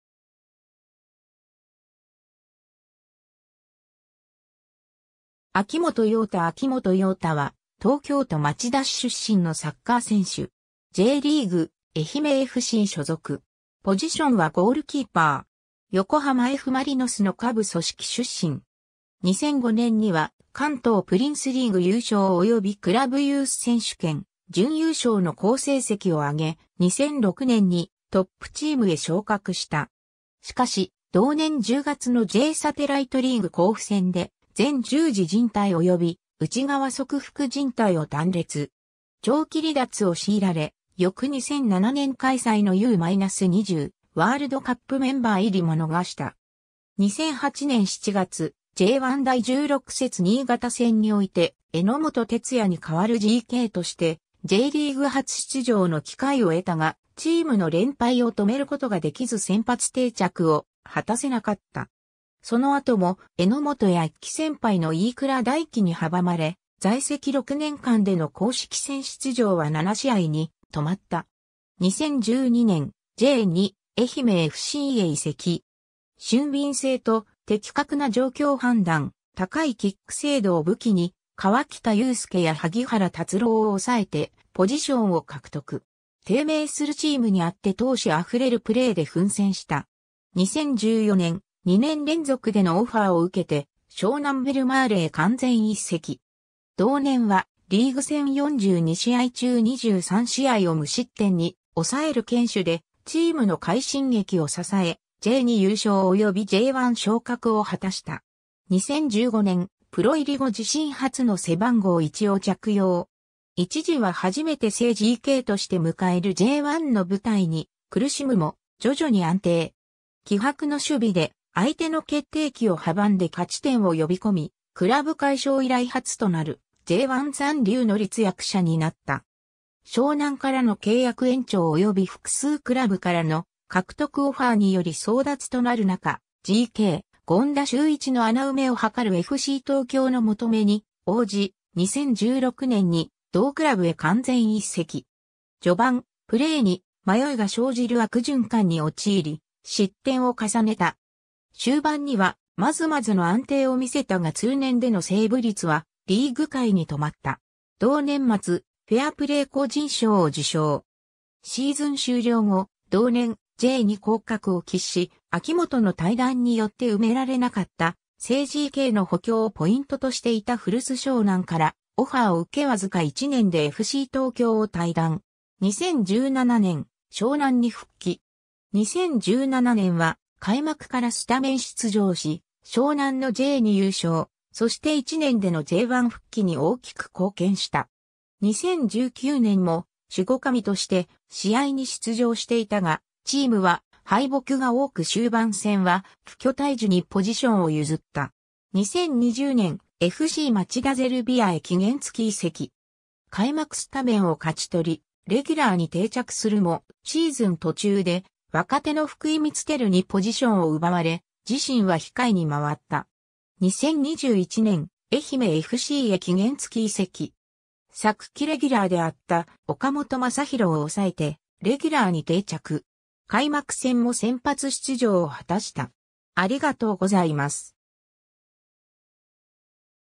秋元陽太
横浜F・マリノスの選手
秋元陽太 秋元 陽太（あきもと ようた、1987年7月11日 - ）は、東京都町田市出身のサッカー選手。
Jリーグ・愛媛FC所属。
ポジションはゴールキーパー（GK）。
 横浜F・マリノスの下部組織出身。
2005年には関東プリンスリーグ優勝及びクラブユース選手権準優勝の好成績を挙げ、2006年にトップチームへ昇格した。
しかし同年10月のJサテライトリーグ甲府戦で前十字靭帯及び内側側副靱帯を断裂。
長期離脱を強いられ、翌2007年開催のU-20ワールドカップメンバー入りも逃した。
2008年7月、J1第16節新潟戦において榎本哲也に代わるGKとしてJリーグ初出場の機会を得たが、チームの連敗を止めることができず先発定着を果たせなかった。
その後も榎本や1期先輩の飯倉大樹に阻まれ、在籍6年間での公式戦出場は7試合に留まった。
 2012年、J2・愛媛FCへ移籍。
俊敏性と的確な状況判断、高いキック精度を武器に川北裕介や萩原達郎を抑えてポジションを獲得。
低迷するチームにあって闘志溢れるプレーで奮戦した。
 2014年、2年連続でのオファーを受けて湘南ベルマーレへ完全移籍。
同年はリーグ戦42試合中23試合を無失点に抑える堅守でチームの快進撃を支え、J2優勝及びJ1昇格を果たした。
2015年、プロ入り後自身初の背番号1を着用。
一時は初めて正GKとして迎えるJ1の舞台に苦しむも、徐々に安定。
気迫の守備で相手の決定機を阻んで勝ち点を呼び込み、クラブ改称以来初となるJ1残留の立役者になった。
 湘南からの契約延長及び複数クラブからの獲得オファーにより争奪となる中、GK権田修一の穴埋めを図るFC東京の求めに応じ、2016年に同クラブへ完全移籍。
序盤、プレーに迷いが生じる悪循環に陥り失点を重ねた。
終盤にはまずまずの安定を見せたが通年でのセーブ率はリーグ下位に留まった。
同年末、フェアプレー個人賞を受賞。
シーズン終了後、同年J2降格を喫し、秋元の退団によって埋められなかった正GKの補強をポイントとしていた古巣湘南からオファーを受けわずか1年でFC東京を退団。
2017年、湘南に復帰。
 2017年は開幕からスタメン出場し、湘南のJ2優勝、そして1年でのJ1復帰に大きく貢献した。
 2019年も守護神として試合に出場していたが、チームは敗北が多く終盤戦は富居大樹にポジションを譲った。
 2020年、FC町田ゼルビアへ期限付き移籍。
開幕スタメンを勝ち取り、レギュラーに定着するも、シーズン途中で若手の福井光輝にポジションを奪われ、自身は控えに回った。
 2021年、愛媛FCへ期限付き移籍。
 昨季レギュラーであった岡本昌弘を抑えてレギュラーに定着。
開幕戦も先発出場を果たした。